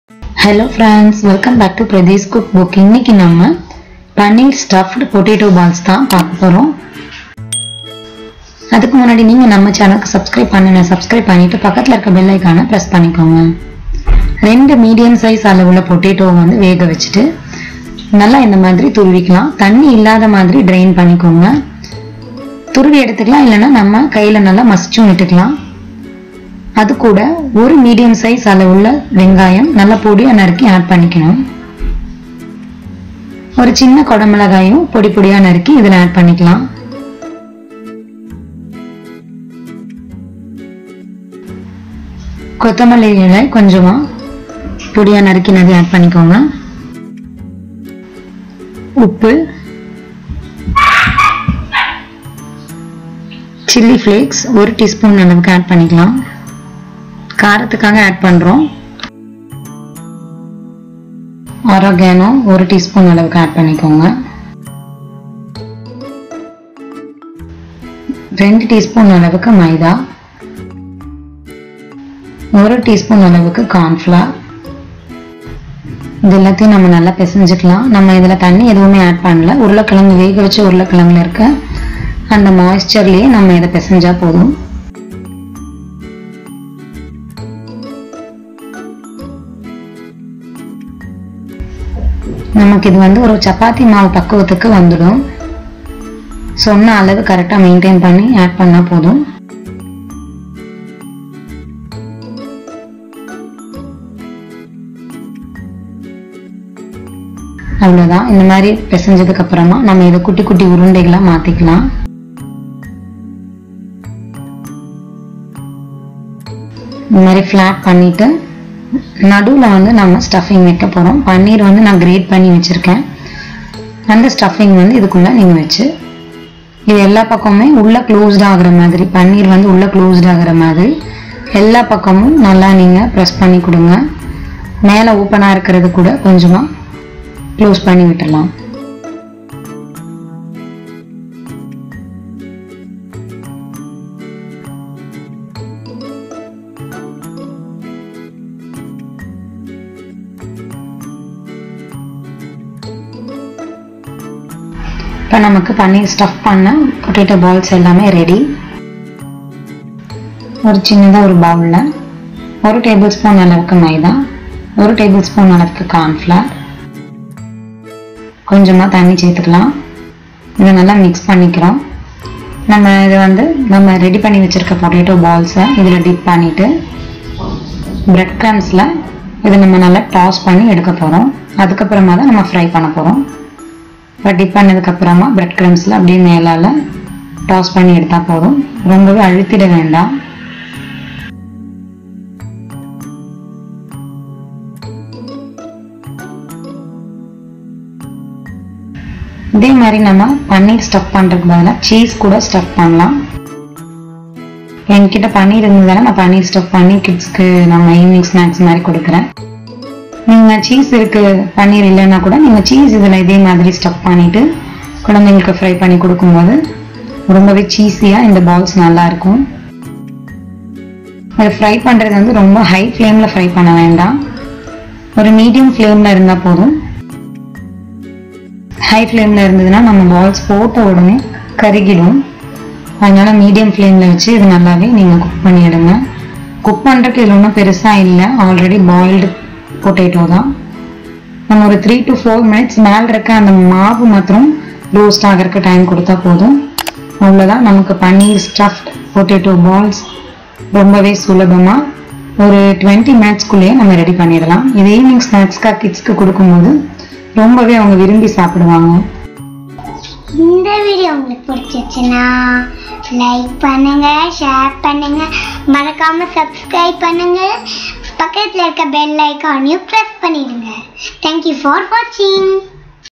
Paneer Potato Balls Recipe|Aloo Paneer Balls|Paneer Stuffed Potato Balls|Paneer Potato Ball in Tamil
Welcome to Pradee's Cook Book. Today we are going to see Paneer Potato Balls Recipe. This is a quick indian vegetarian snacks recipe and also very simple to make for your guest or kids as a evening snacks. It tasted very yummy so try this Paneer Potato Balls recipe and share your feedbacks in the comment section.

INGREDIENT:
Boiled Potato - 2 no's
Onion medium size - 1 no
Capsicum small size - 1 no
Coriander leaves chopped - few
Cornflour - 1 tbsp [for balls dipping]
Bread crumbs
Paneer